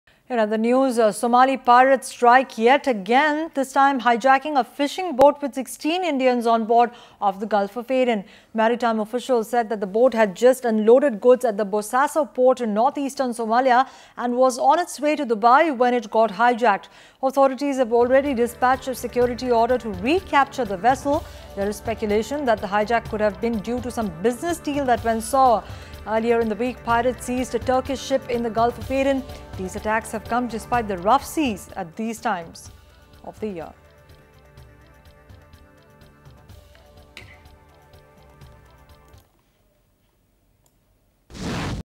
Somali pirates hijack Indian dhow
The Somali pirates strike yet again, this time hijacking an Indian dhow with 16 crew onboard as it left the north coast port of Bossaso, in the Gulf of Aden. Download the Times Now India's Election HQ app and get all the election info at one go. Social Media Links